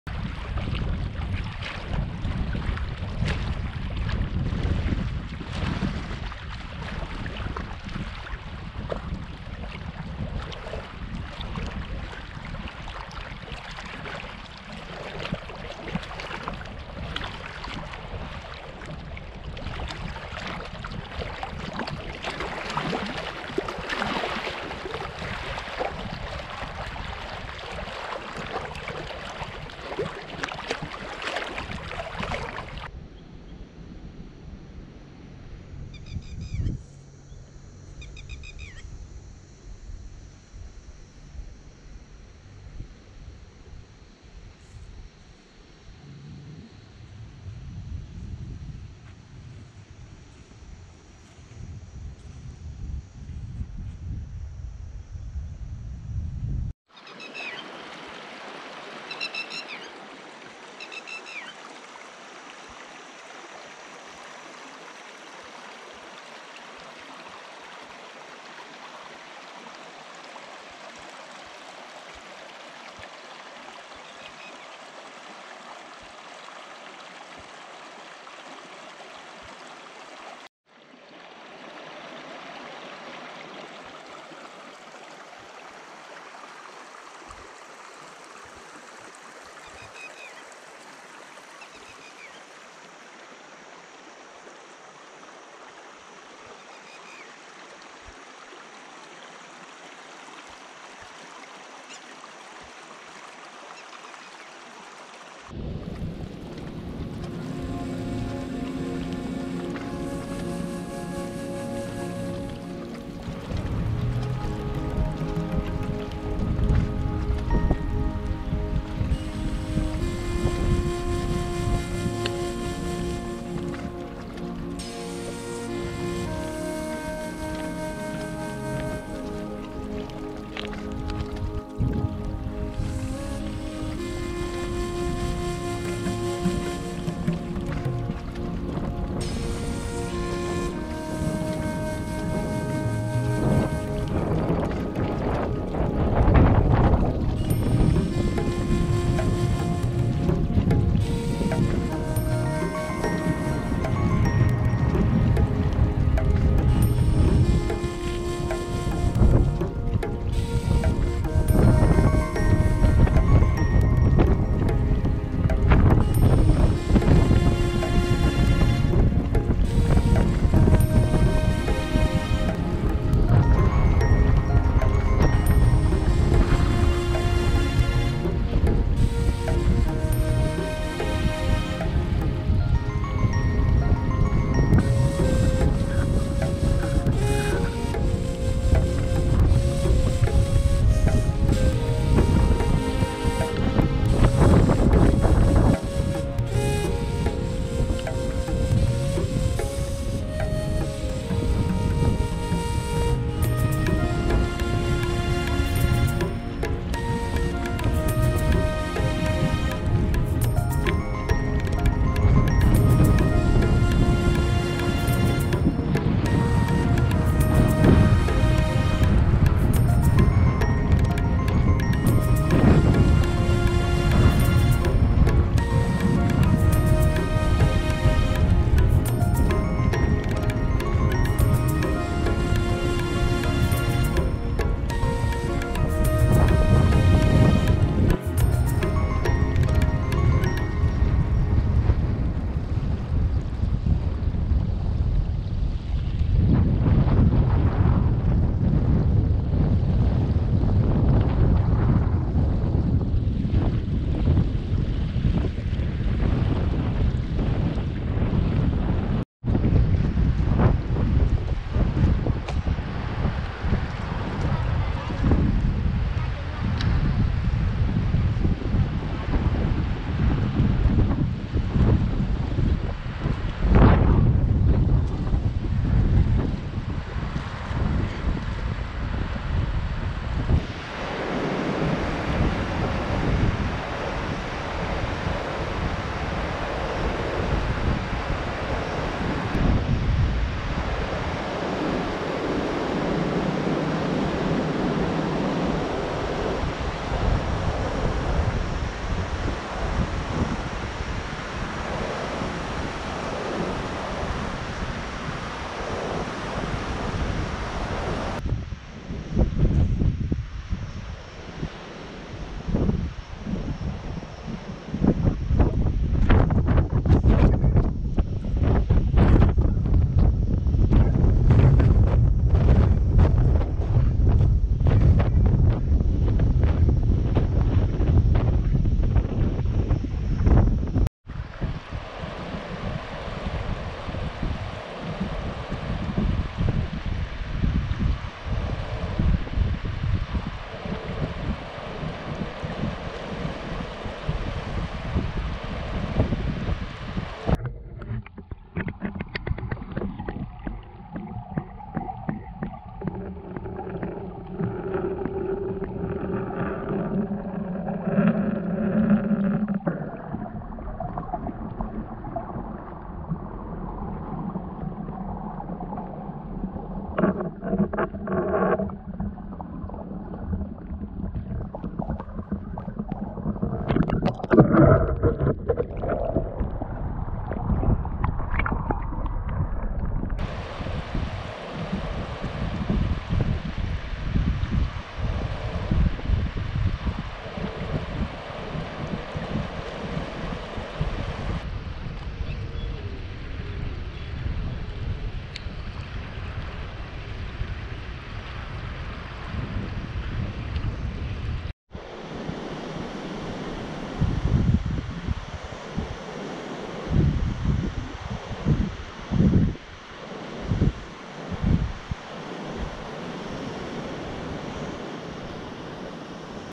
සිඳීගිය රන්දෙණිගල ජලාශයෙන් මතුවූ පැරණි මහවැලි ගඟ සහ වෙල්යාය| Randenigala at Draught in 2023
1986 වසරේ වැඩ අවසන් කරන ලද ,මහවැලි ව්‍යාපාරය යටතේ ඉදිකරනා ලද විශාලතම ජලාශයවන රන්දෙණිගල ජලාශය,මේ දිනවල (2023 ජූලි/අගෝස්තු) පවතින දැඩි නියං සහිත කාලගුණ තත්ත්වය නිසා, එහි වූ ජල මට්ටම බොහෝ සෙයින් පහත බැස ඇත.මේ හේතුවෙන් ජලාශය ඉදිකිරීමේදී එයට යටවූ කඳු,කුඹුරු සහ පැරණි මහවැලි ගංගාව මේ දිනවල නැවත දැකගත හැක.